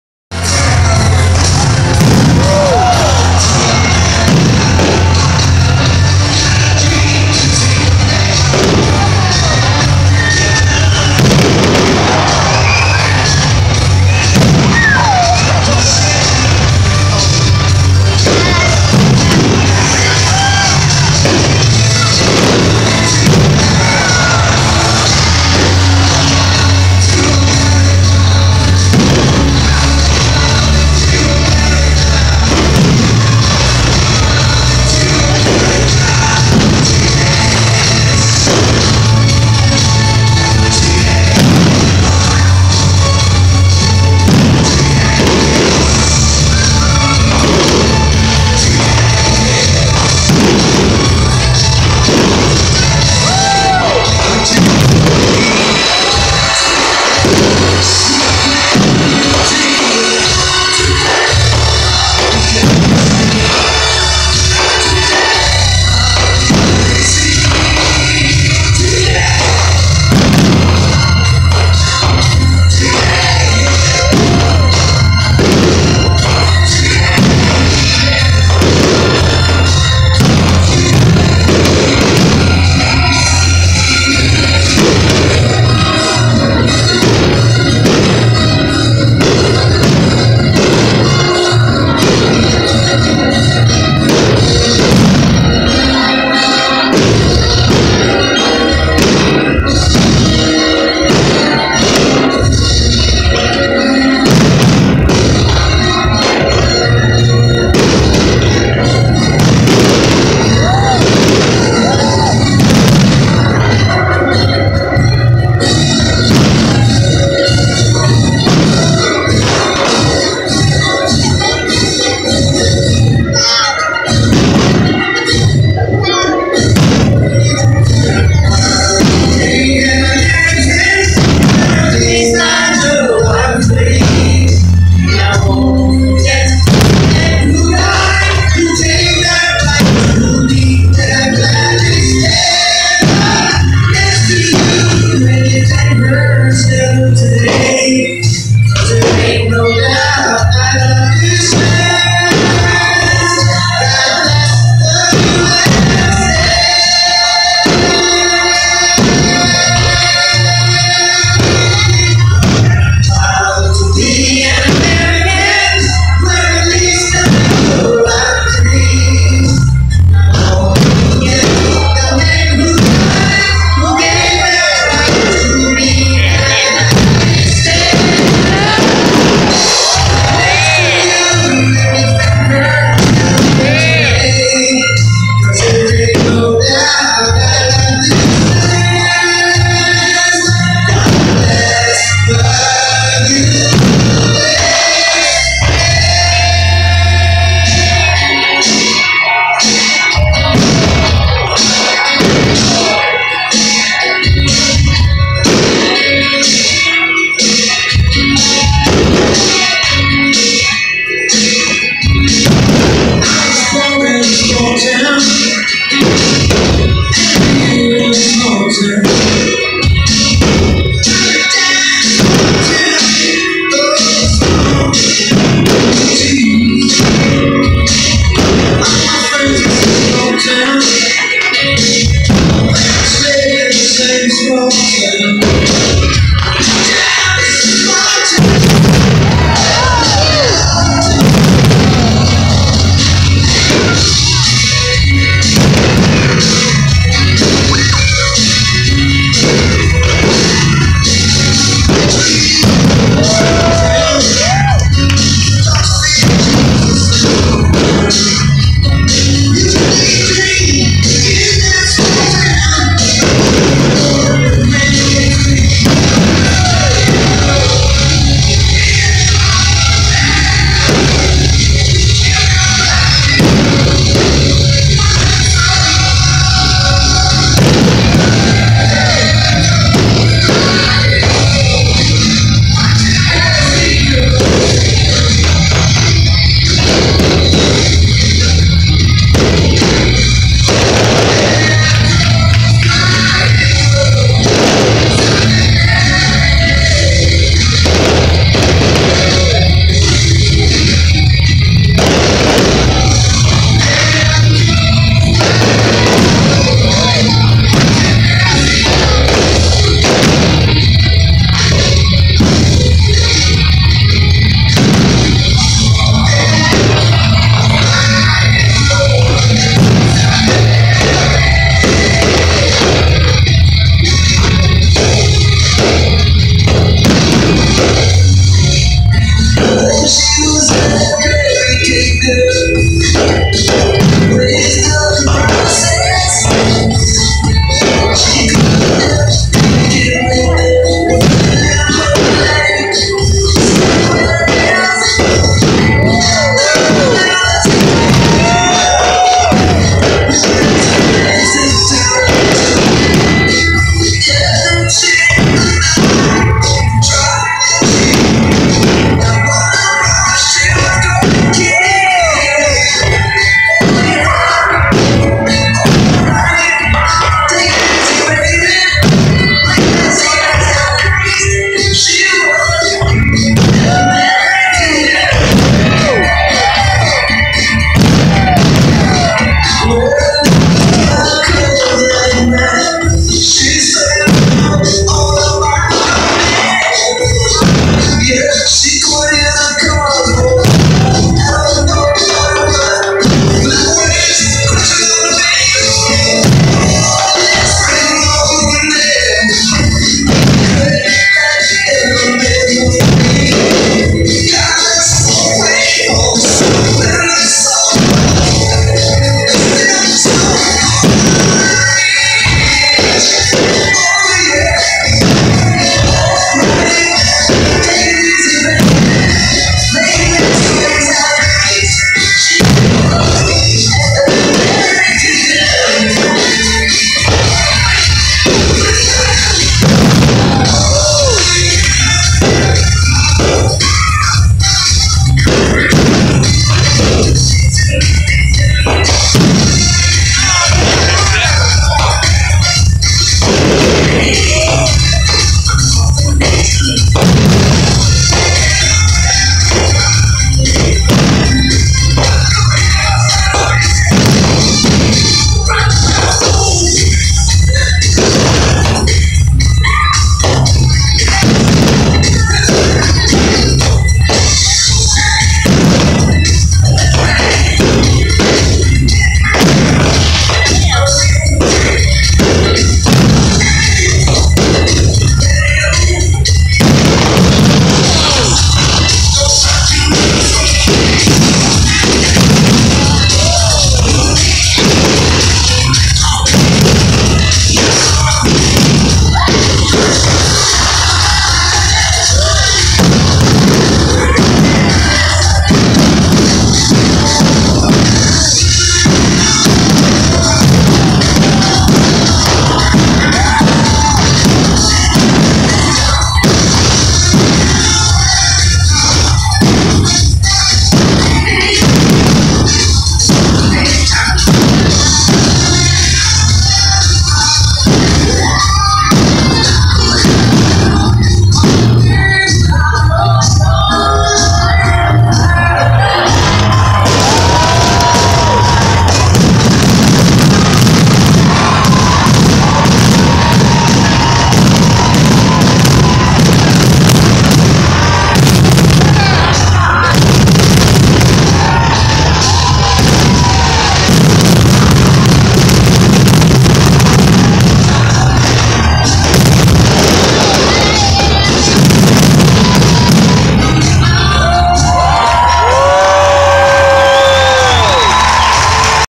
Happy 4th of July!!
Fourth of July firework celebration at the Jacksonville Jumbo Shrimp game. (Sorry for the blurry beginning)